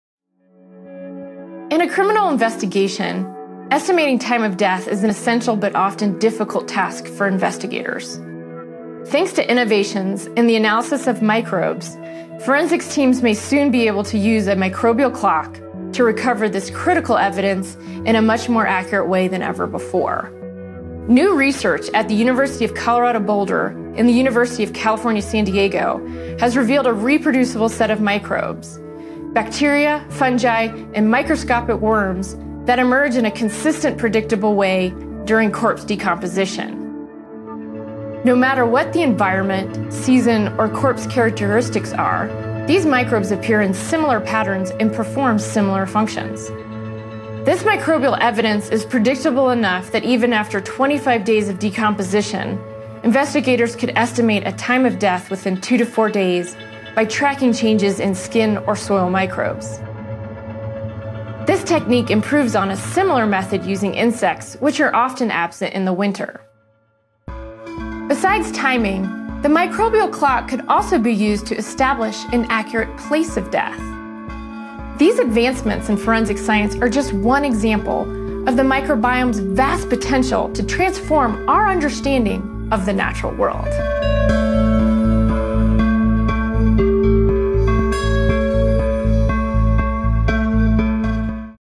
Murder investigators could use microbes as evidence: CU-Boulder Study
In a criminal investigation, establishing time of death is an essential but often difficult task for investigators. Thanks to innovations in the analysis of microbes, forensic teams may soon be able to use a “microbial clock” to recover this critical evidence in a much more accurate way than ever before.